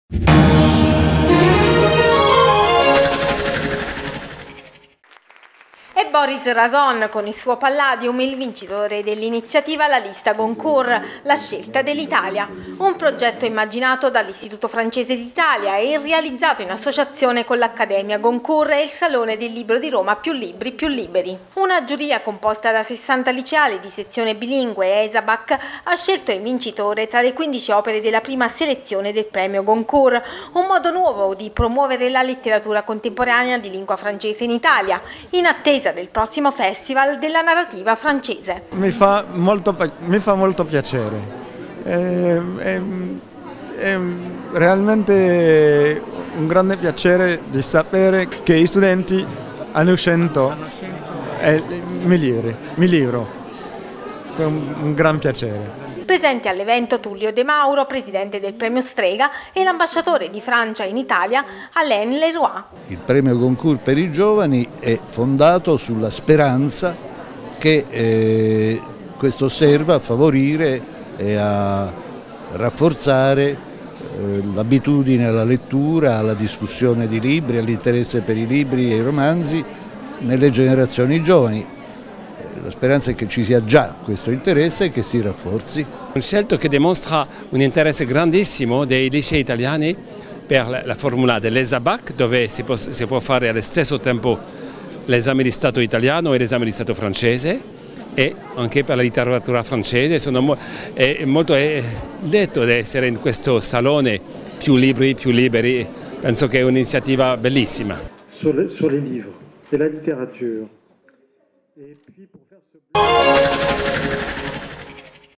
Premio Goncourt a Boris Razon, a Più libri più liberi il riconoscimento per la letteratura
È Boris Razon con il suo "Palladium" il vincitore dell'iniziativa "La lista Goncourt, la scelta dell'Italia', un progetto immaginato dall'Institut français d'Italia, e realizzato in associazione con l'Accademia Goncourt e il salone del libro di Roma "Più libri più liberi".